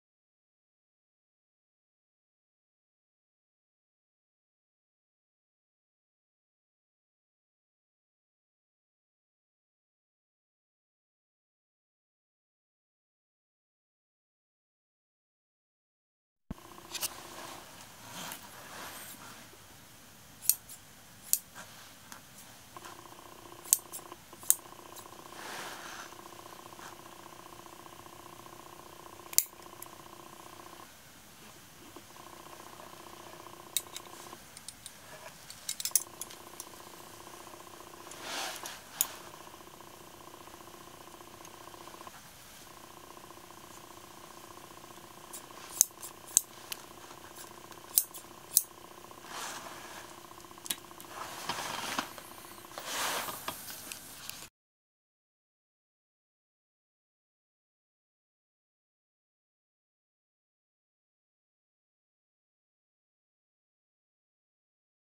鳥部製作所 新型キッチンスパッター KS-203
オールステンレスキッチン鋏
取り外し可能で食洗機OK!